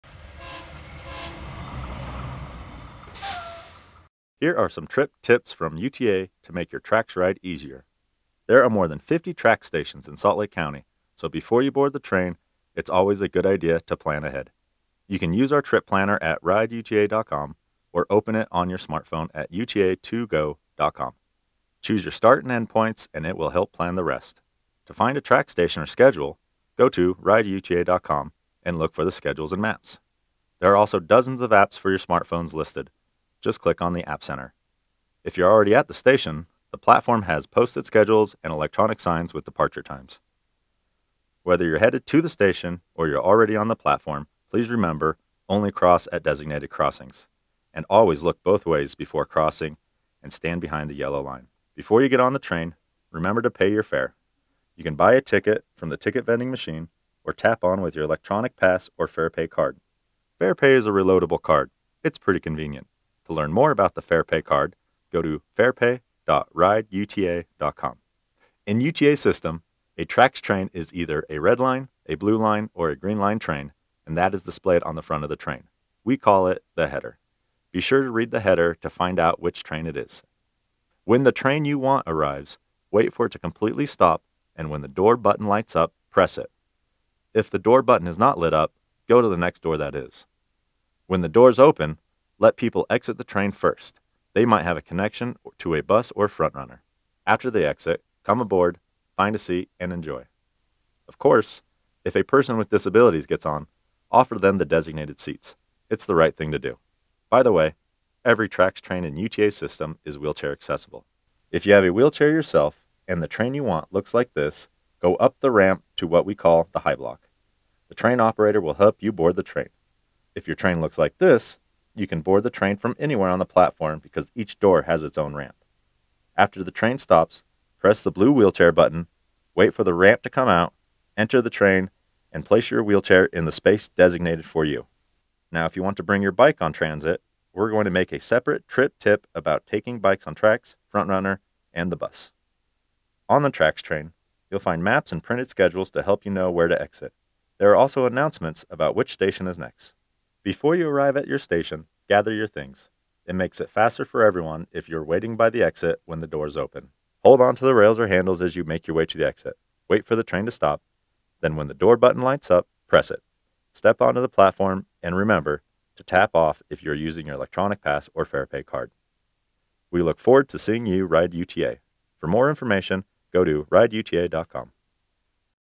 How to Ride TRAX
UTA's lightrail system, TRAX, makes it easy to travel to work, school, events and more without the hassle and expense of driving. Learn everything you need to know about riding TRAX in this short video.